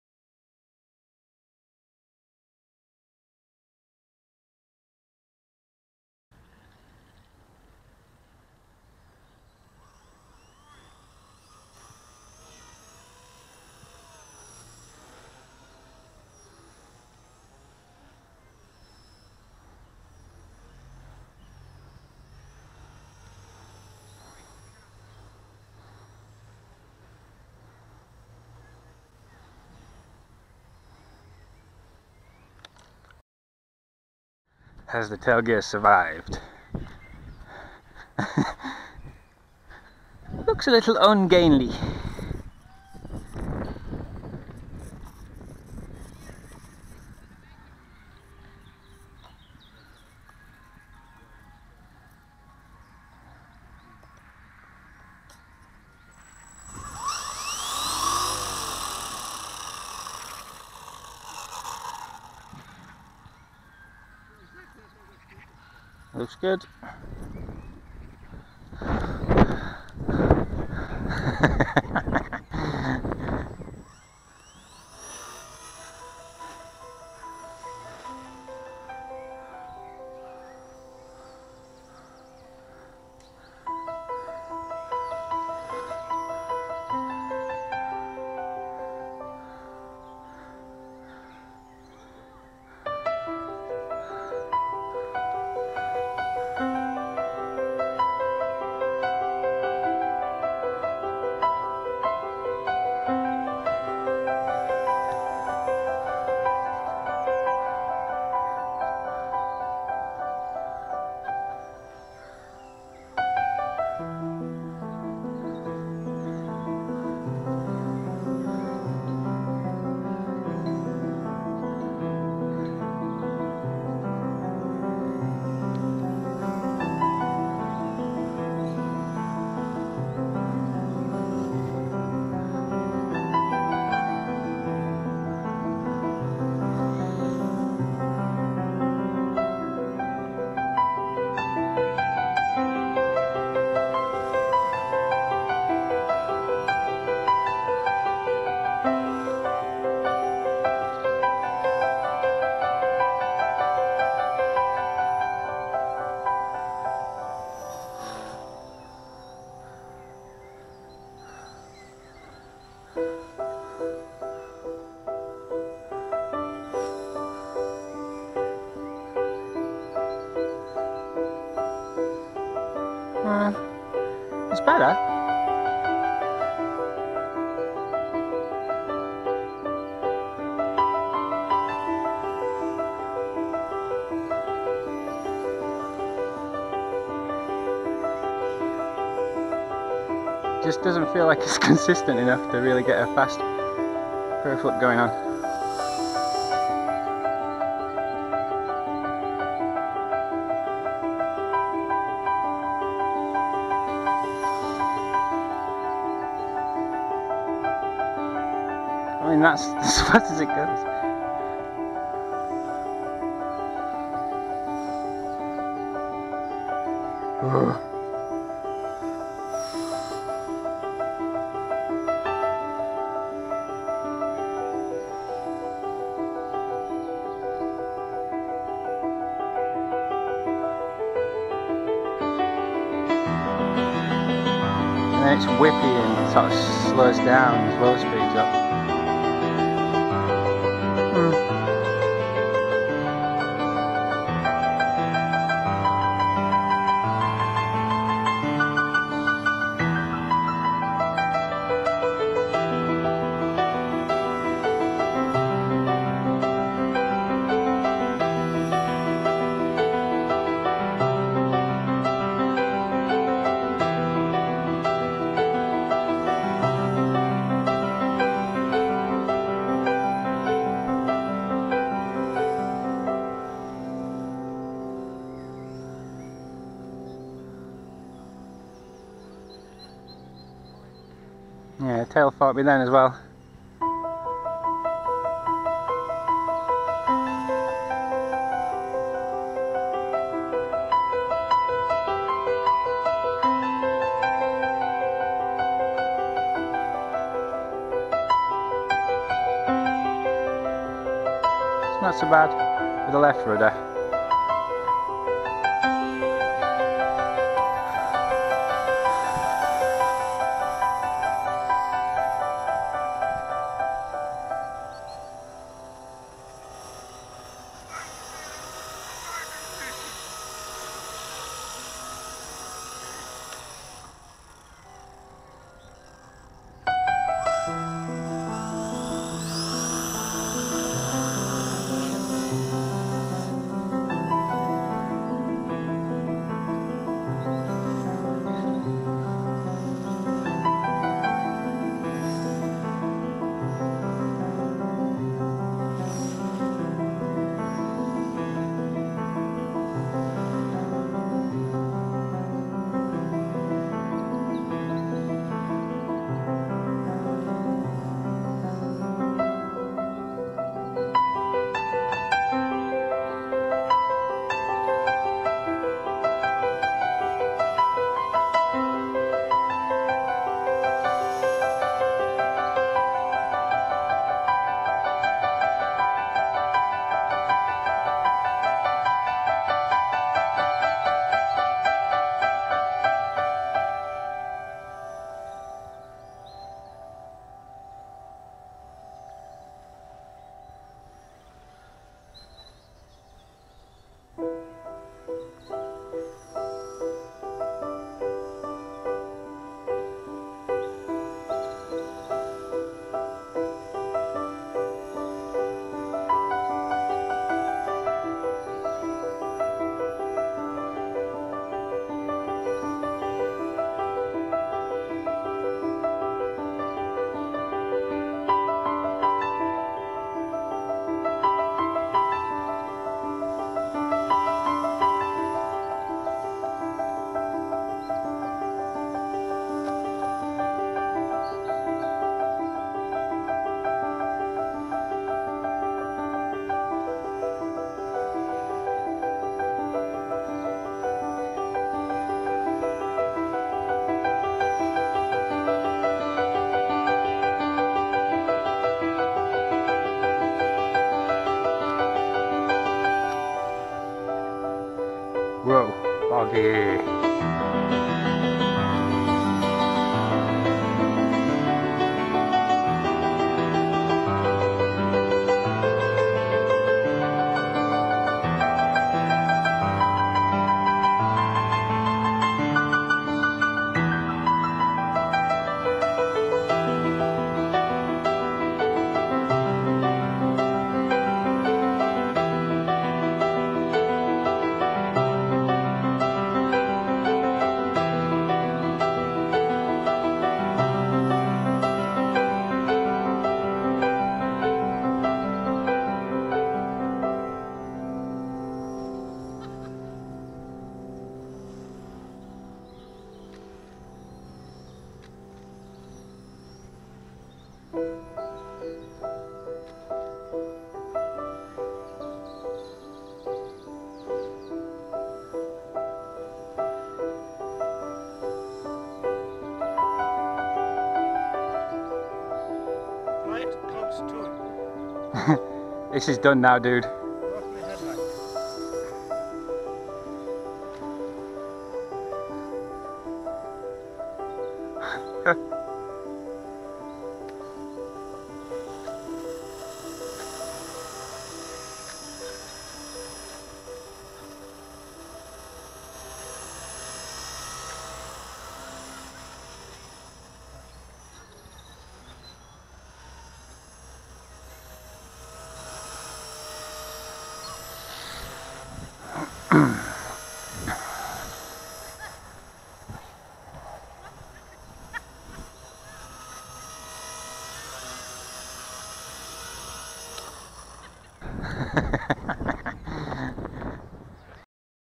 V120D02S stock - maximum rates and no expo
Cyclic is as quick as I can get it with stock blades and the tail still needs tuning better but I really enjoyed this couple of flights all the same. First small tumble today over estimating the angle on an inverted hurricane turning back into the wind but but no damage to tail gears due to early TH. I will fit a slipper clutch but I need to tune the tail first.